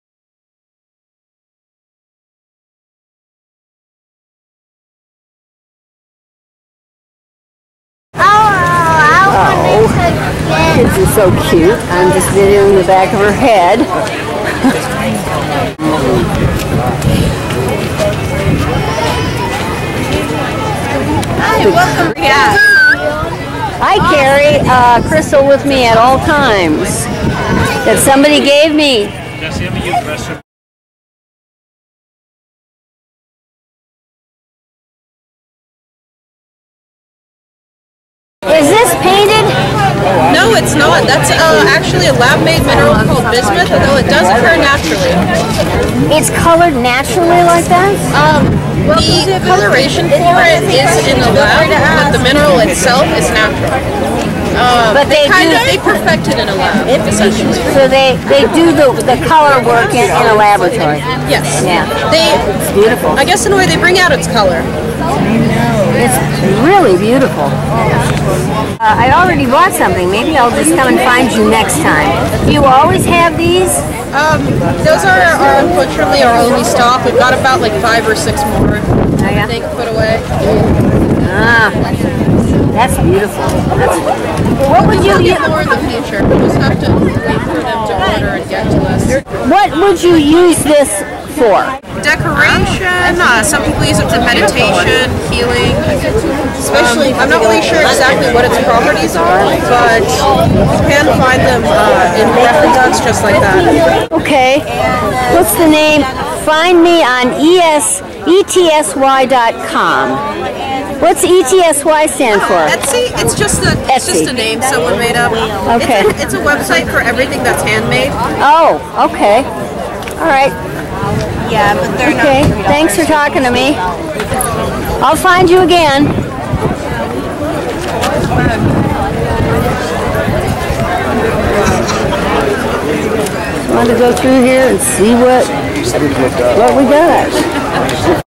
5 - A BISMUTH CRYSTAL - TREASURE ISLAND FLEA - 5-31-15
This crystal was artificially created in a lab. I would have enjoyed watching as the stepped shapes slowly formed from it.  The lovely rainbow of metallic colors was created by a thin oxide layer on the bismuth's surface refracting the rays of light shining through it.   Bismuth is slightly radioactive.  How appropriate for a radioactive isle.  Do I want to use a hot object for meditation?  Bismuth's unstable atoms take from just a few hours to 8 million years to undergo radioactive decay. That's the oldest known calculated age of the Cosmos.